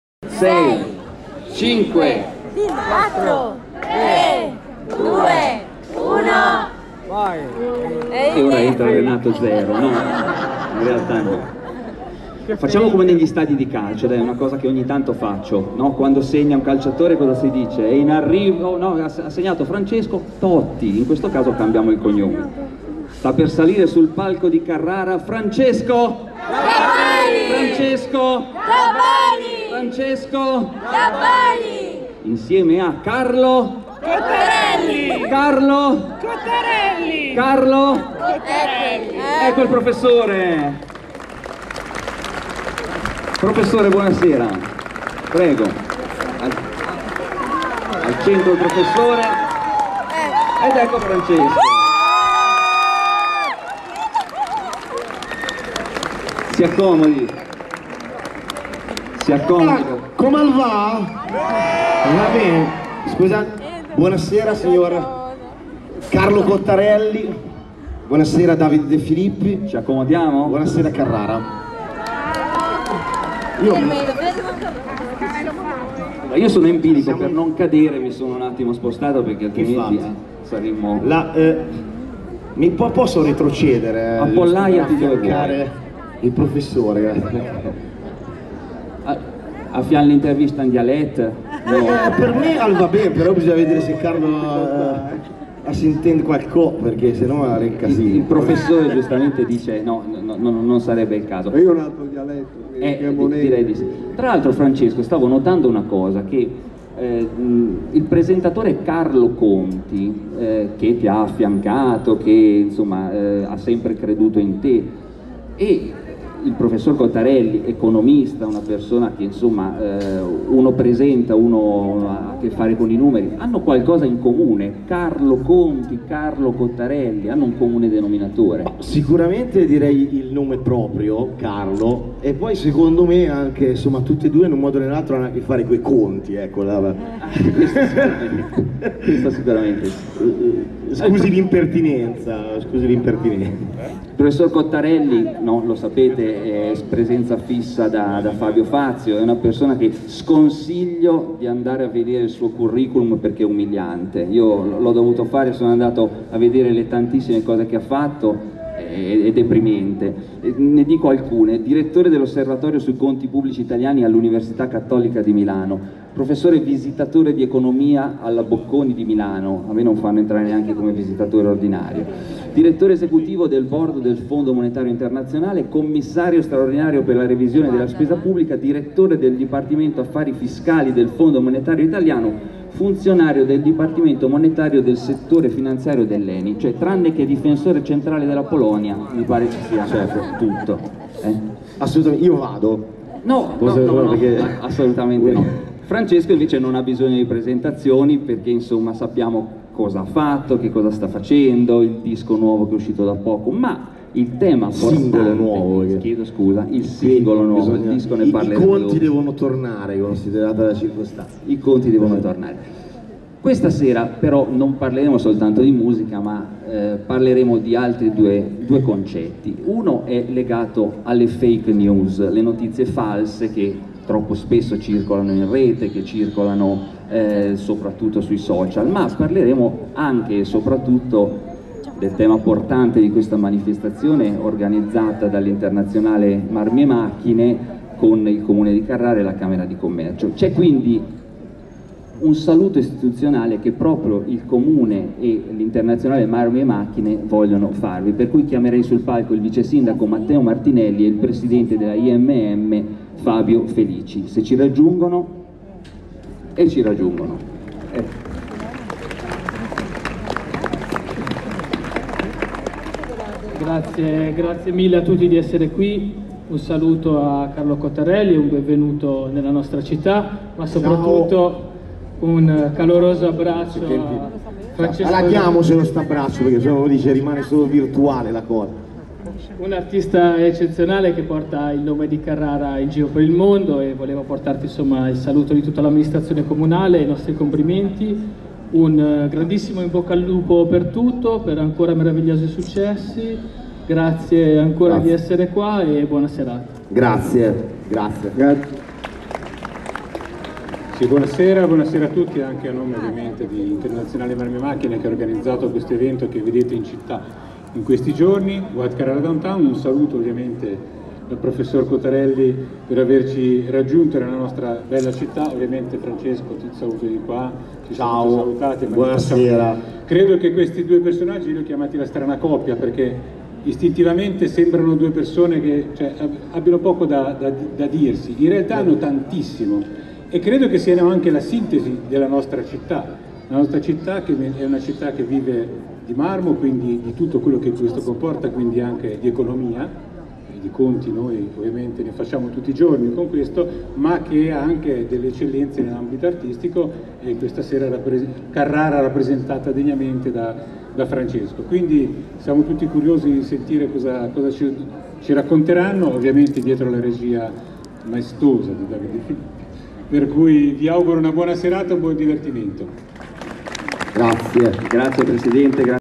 FRANCESCO GABBANI - 1° PARTE - Carlo Cottarelli - Presentato da David Di Filippi
White Carrara Downtown - Marble Cafè
3 Giugno 2019-  Carrara